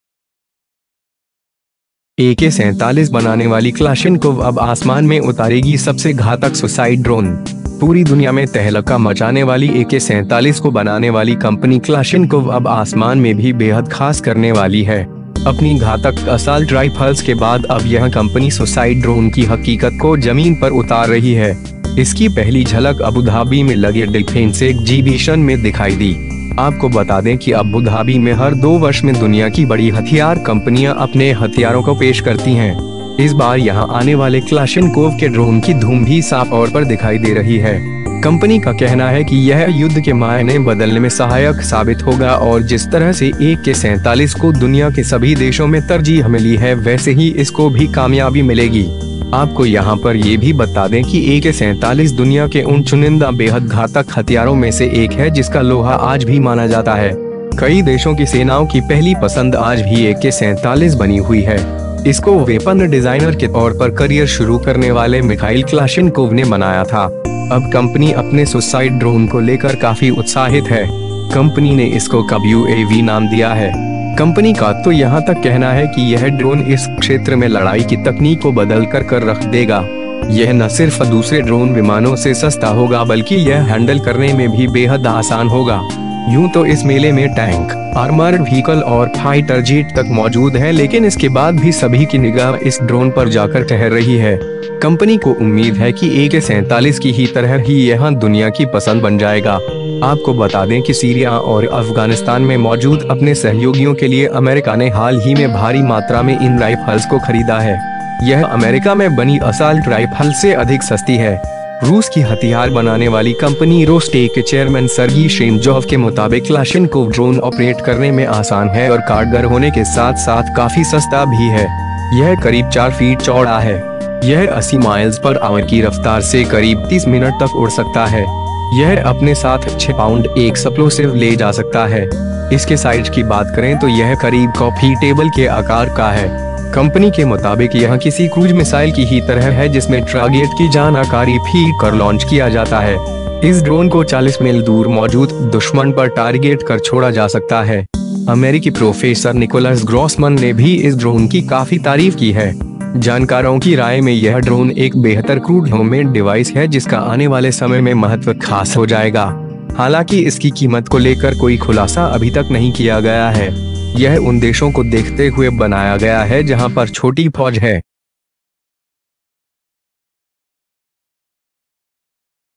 Ak 47 बनाने वाली क्‍लाशिनकोव अब आसमान में उतारेगी सबसे घातक सुसाइड ड्रोन
पूरी दुनिया में तहलका मचाने वाली AK-47 को बनाने वाली कंपनी क्‍लाशिनकोव अब आसमान में भी बेहद खास करने वाली है। अपनी घातक असाल्ट राइफल्‍स के बाद अब यह कंपनी सुसाइड ड्रोन की हकीकत को जमीन पर उतार रही है। इसकी पहली झलक अबुधाबी में लगे डिफेंस एग्‍जीबीशन में दिखाई दी। आपको बता दें कि अबु धाबी में हर दो वर्ष में दुनिया की बड़ी हथियार कंपनियां अपने हथियारों को पेश करती हैं। इस बार यहां आने वाले क्‍लाशिनकोव के ड्रोन की धूम भी साफतौर पर दिखाई दे रही है। कंपनी का कहना है कि यह युद्ध के मायने बदलने में सहायक साबित होगा और जिस तरह से AK-47 को दुनिया के सभी देशों में तरजीह मिली है वैसे ही इसको भी कामयाबी मिलेगी। आपको यहां पर ये भी बता दें कि AK-47 दुनिया के उन चुनिंदा बेहद घातक हथियारों में से एक है जिसका लोहा आज भी माना जाता है। कई देशों की सेनाओं की पहली पसंद आज भी AK-47 बनी हुई है। इसको वेपन डिजाइनर के तौर पर करियर शुरू करने वाले मिखाइल क्‍लाशिनकोव ने बनाया था।

अब कंपनी अपने सुसाइड ड्रोन को लेकर काफी उत्‍साहित है। कंपनी ने इसको KUB-UAV नाम दिया है। कंपनी का तो यहां तक कहना है कि यह ड्रोन इस क्षेत्र में लड़ाई की तकनीक को बदलकर कर रख देगा। यह न सिर्फ दूसरे ड्रोन विमानों से सस्‍ता होगा बल्कि यह हैंडल करने में भी बेहद आसान होगा। यूं तो इस मेले में टैंक, आर्मर्ड व्‍हीकल और फाइटर जेट तक मौजूद हैं लेकिन इसके बाद भी सभी की निगाह इस ड्रोन पर जाकर ठहर रही है।

कंपनी को उम्‍मीद है कि एके 47 की ही तरह ही यह दुनिया की पसंद बन जाएगा। आपको बता दें कि सीरिया और अफगानिस्‍तान में मौजूद अपने सहयोगियों के लिए अमेरिका ने हाल ही में भारी मात्रा में इन राइफल्‍स को खरीदा है। यह अमेरिका में बनी असाल्‍ट राइफल्‍स से अधिक सस्‍ती है।

रूस की हथियार बनाने वाली कंपनी रोस्‍टेक के चेयरमेन सर्गी शेमजॉव के मुताबिक क्‍लाशिनकोव ड्रोन ऑपरेट करने में आसान है और कारगर होने के साथ-साथ काफी सस्‍ता भी है। यह करीब चार फीट चौड़ा है। यह 80 mph की रफ्तार से करीब 30 मिनट तक उड़ सकता है। यह अपने साथ छह पाउंड एक्‍सप्‍लोसिव ले जा सकता है। इसके साइज की बात करें तो यह करीब कॉफी टेबल के आकार का है। कंपनी के मुताबिक यह किसी क्रूज मिसाइल की ही तरह है जिसमें ट्रागेट की जानाकारी फीड कर लॉन्‍च किया जाता है।

इस ड्रोन को चालीस मील दूर मौजूद दुश्‍मन पर टार्गेट कर छोड़ा जा सकता है। अमेरिकी प्रोफेसर निकोलस ग्रॉसमन ने भी इस ड्रोन की काफी तारीफ की है। जानकारों की राय में यह ड्रोन एक बेहतर क्रूड होममेड डिवाइस है जिसका आने वाले समय में महत्‍व खास हो जाएगा। हालांकि इसकी कीमत को लेकर कोई खुलासा अभी तक नहीं किया गया है। यह उन देशों को देखते हुए बनाया गया है जहां पर छोटी फौज है।